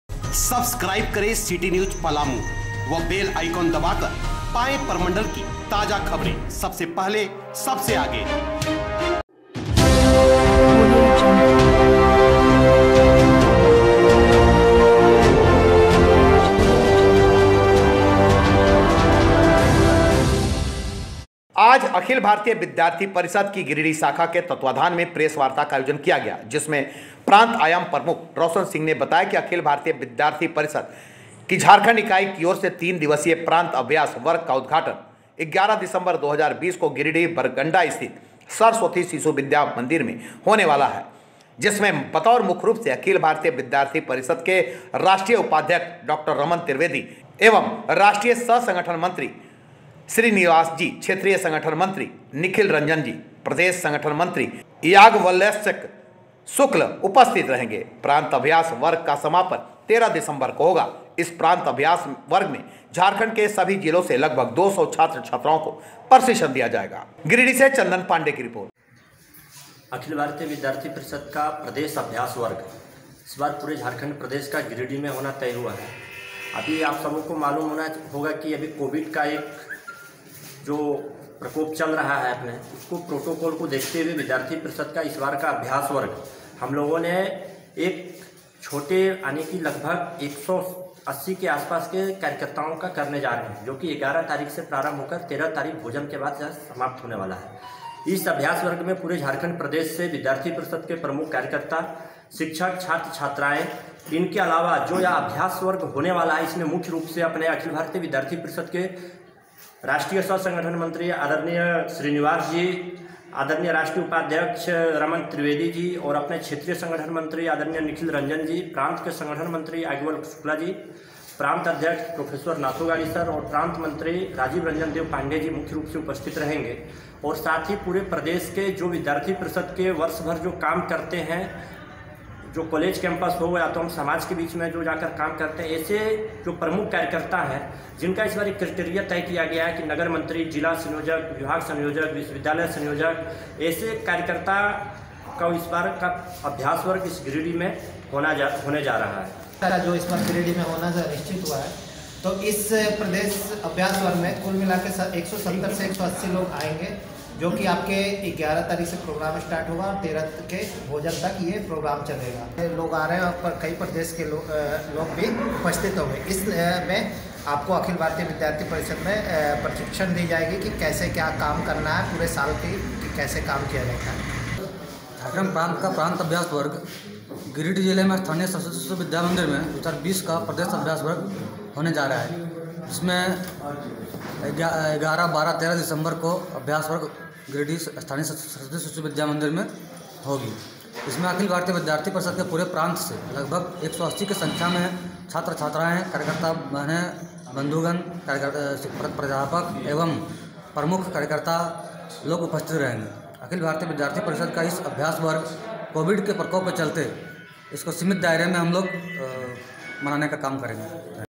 गिरिडीह ---विद्यार्थी परिषद् का 11 दिसंबर से शुरू होगा तीन दिवसीय प्रान्त अभ्यास वर्ग
चन्दन पांडेय की रिपोर्ट